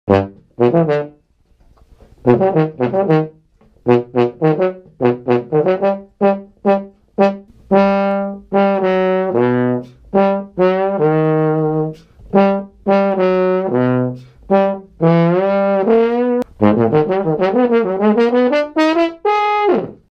Jhosir Cordova - Trombone Recording - Tania de Venezuela
Jhosír Córdova: Trombone recording for Tania of Venezuela.
Latin / Cumbia / Merengue